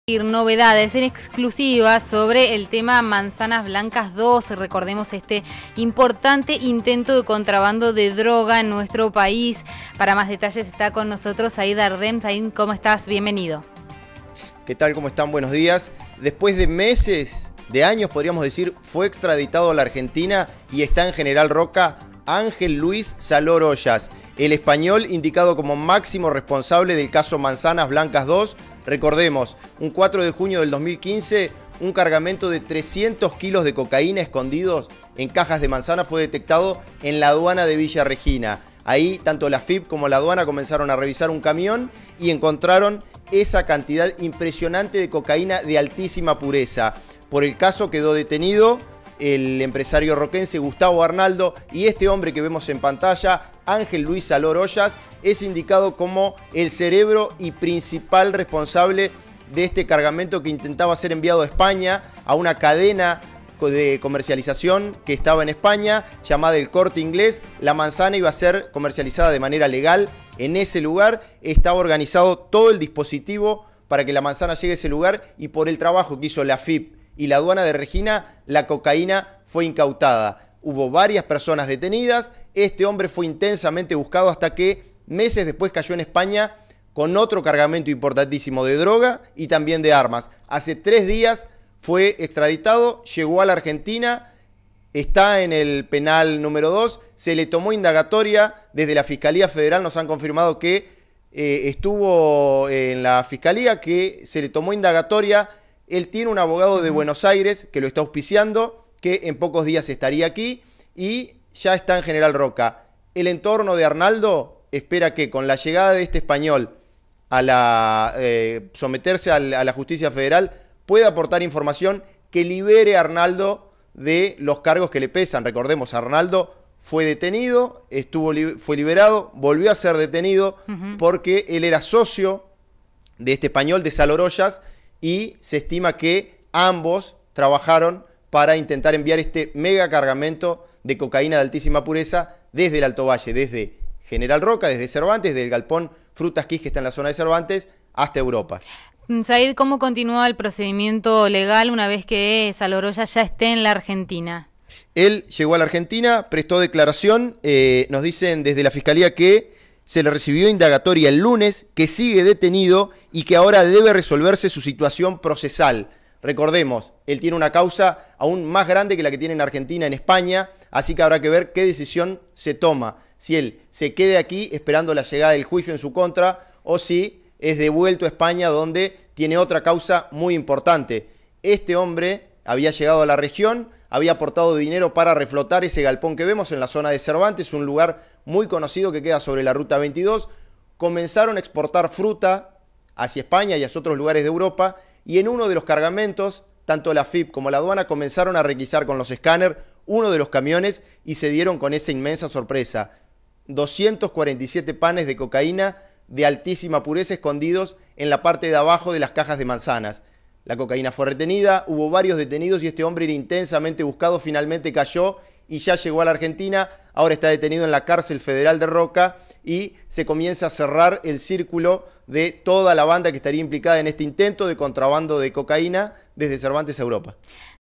Manzanas blancas II: Salor Hoyas detenido en Roca
El español indicado como líder del caso manzanas blancas II, Ángel Luis Salor Hoyas, fue extraditado y ya se encuentra en General Roca. Llegó hace pocos días y ya declaró en la justicia federal.